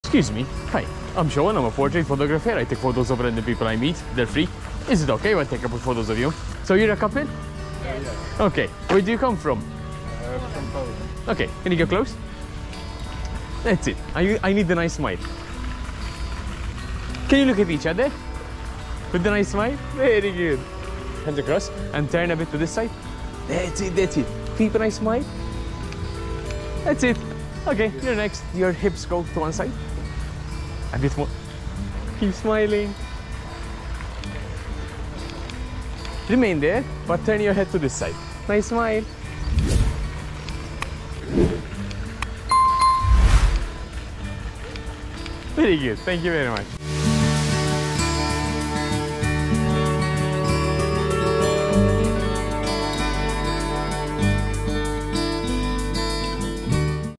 Street photography couple from Poland
Wow!! I met these happy, gorgeous, pretty Polish couple while shooting some street photography in Valletta, Malta. I asked for some photos and they immediately agreed. they where very happy and had magnificent smiles. I started with a group photo and after shot some portraits each. I gave them some posing tips to make them look good although they where very photogenic. They really like posing so I gave them some posing tips during the photoshoot.
The photos turned out great and short video of the street photography in POV style. I also gave several street photography tips during the shoot.

GoPro Hero 11
Latest GoPro Hero 12
Sony A7Riv
Sony 85 F1.8
Sony 85mm F1.4
GoPro chest strap
GoPro 3-Way Grip